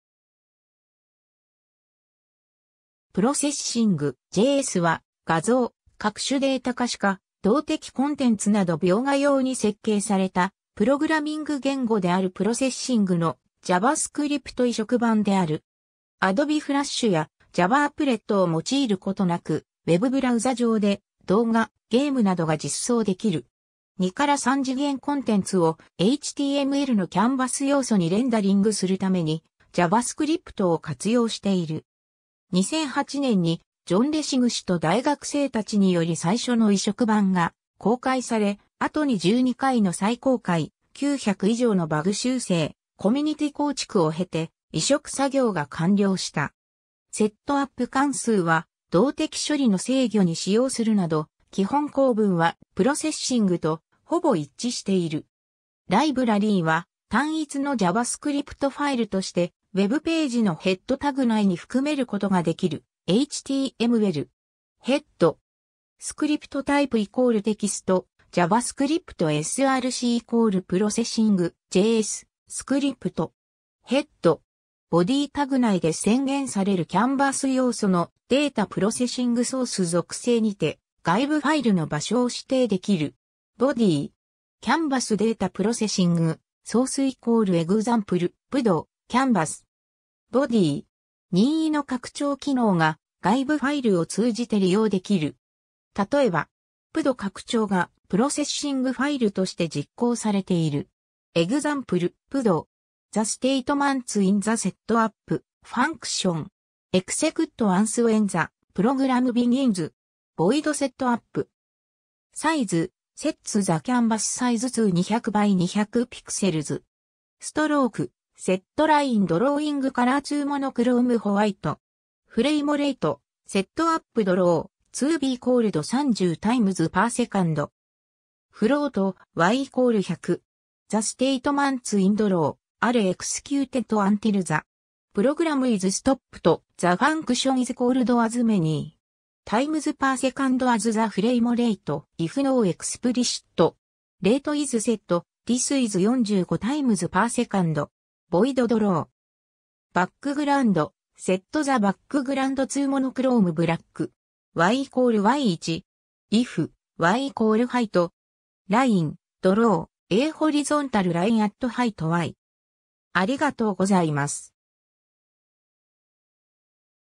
Processing.js
ライブラリ_(プログラミング)
Processing.jsは画像、各種データ可視化、動的コンテンツなど描画用に設計されたプログラミング言語であるProcessingのJavaScript移植版である。
 Adobe FlashやJavaアプレットを用いることなくウェブブラウザ上で動画、ゲームなどが実装できる。
 2-3次元コンテンツをHTMLのcanvas要素（最新バージョンのMozilla Firefox、Opera、Internet Explorer、Safari、Google Chromeなどでサポートしている）にレンダリングするためにJavaScriptを活用している。
 2008年にジョン・レシグ氏と大学生達により最初の移植版が公開され、後に12回の再公開、900以上のバグ修正、コミュニティ構築を経て、移植作業が完了した。
 setup()関数はコンテンツの大きさや画面書換速度などの一般的なプロパティ定義に使用し、 draw()関数は動的処理の制御に使用するなど、基本構文はProcessingとほぼ一致している。
 ライブラリは、単一のJavaScriptファイルとしてWebページのheadタグ内に含めることができる： 「html」
「head」
  「script type="text/javascript" src="processing.js"」「/script」
「/head」
 Bodyタグ内で宣言されるcanvas要素の「data-processing-sources」属性にて、外部ファイルの場所を指定できる。
 「body」
  「canvas data-processing-sources="example.pde"」「/canvas」
「/body」
 任意の拡張機能が外部ファイルを通じて利用できる。
例えば、.pde拡張がProcessingファイルとして実行されている。
 /* example.pde */

// The statements in the setup() function
// execute once when the program begins
void setup()
  size(200, 200);  // Sets the canvas size to 200 by 200 pixels
  stroke(255);     // Set line drawing color to monochrome white
  frameRate(30);   // Set up draw() to be called 30 times per second

float y = 100;

// The statements in draw() are executed until the
// program is stopped. The function is called as many
// times per second as the frameRate. If no explicit
// rate is set, this is 45 times per second.
void draw()
  background(0);   // Set the background to monochrome black
  y = y - 1;
  if (y 「 0) { y = height; }
  line(0, y, width, y);  // draw a horizontal line at height y